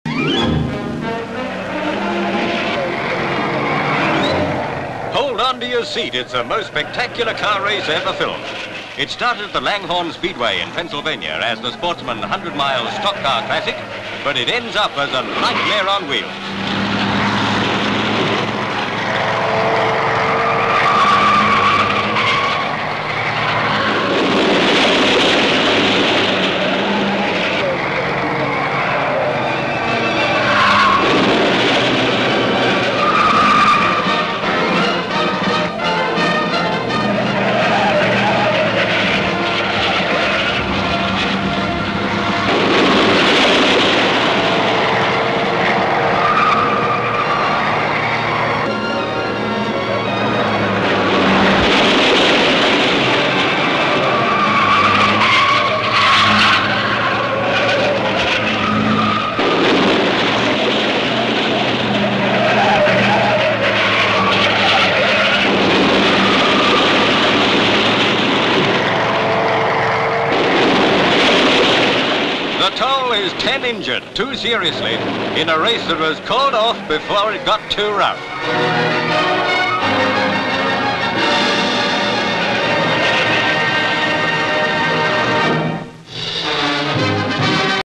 Crack-Ups Stop Big Car Race (1951)
Full title reads: "Crack Ups Stop Big Car Race".

Langhorne Speedway, Pennsylvania, United States of America (USA). Accidents stop a 100 mile stock car race.

LV Pan stock cars racing round track (super title) SV Towards and pan cars rounding bend and into straight. SV Pan car X. SV Pan cars going into corner. LV Towards cars flashing past camera. LV Cars round bend - one crashes. SV Cars passing camera. SV Car crashing. LV Car crashing into wreckage and turns round, rolls across track and blows up, two more cars crash, a third collides with them. SV Pan wrecked and blazing cars. LV  Race in progress past blazing car - car overturns, another crashes into it. Two more collide.

(Orig.F.G.) (Orig."G")
 FILM ID:1477.3